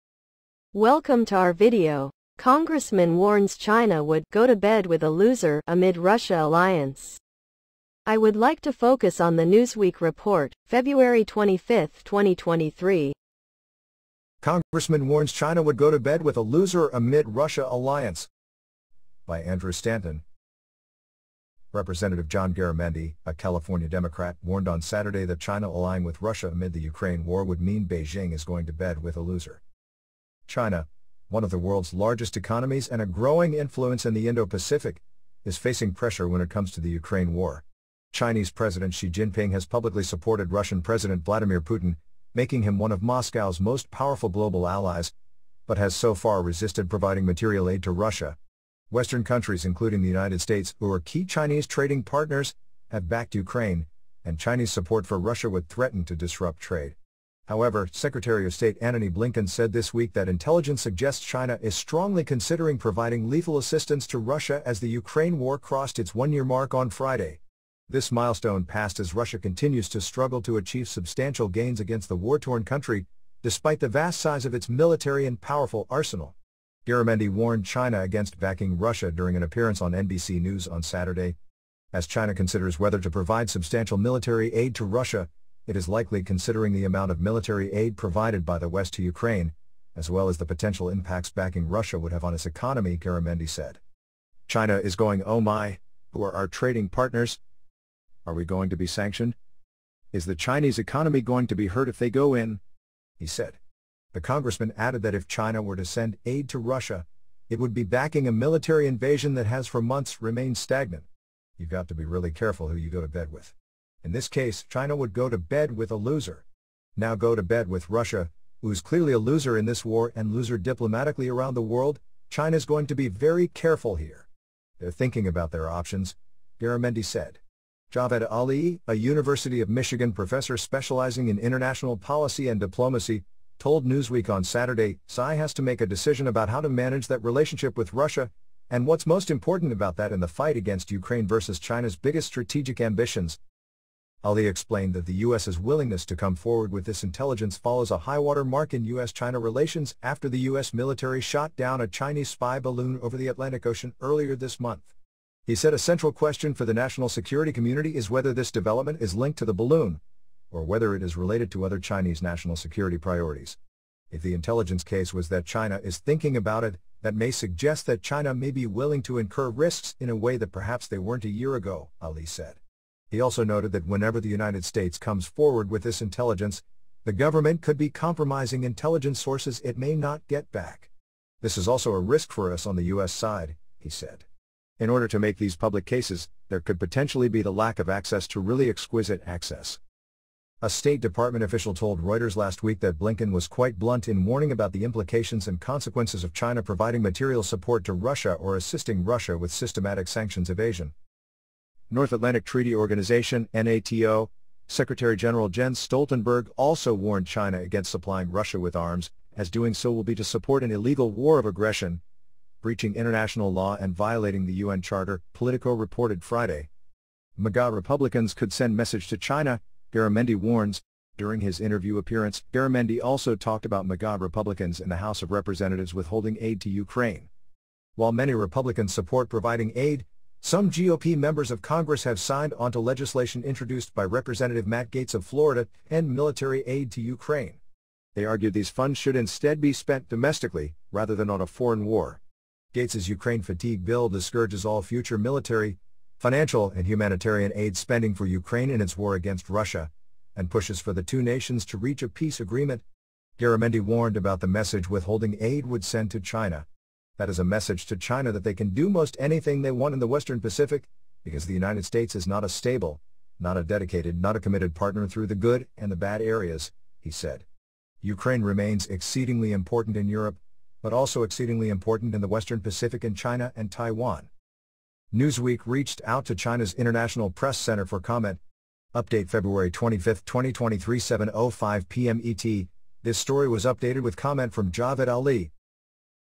Congressman Warns China Would 'Go to Bed With a Loser' Amid Russia Alliance.
Newsweek, February 25, 2023.